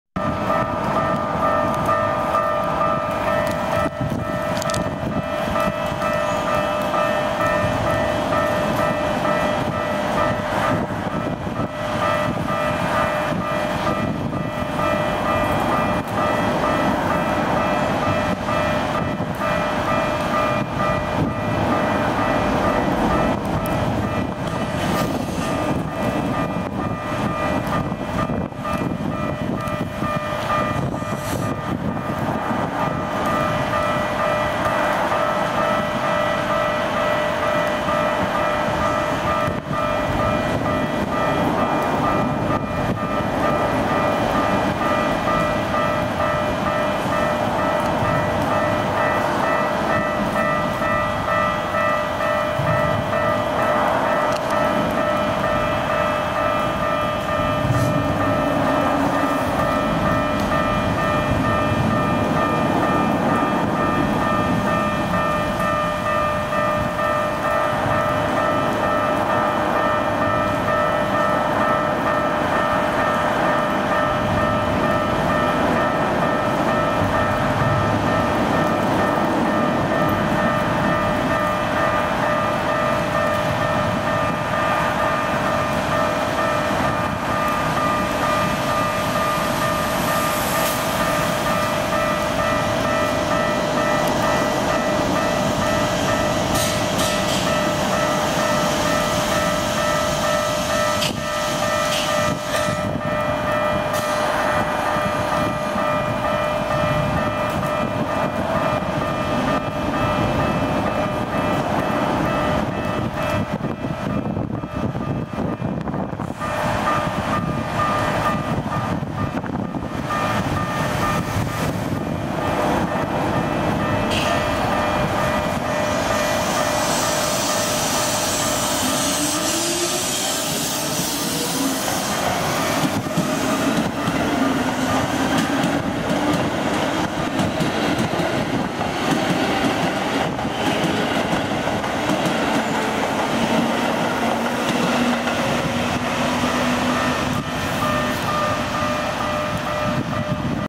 レア！ 北総（千葉ニュータウン鉄道）９０００形 快特 穴守稲荷 発車
１月１８日　オリンパス　EーPL6にて、手持ち撮影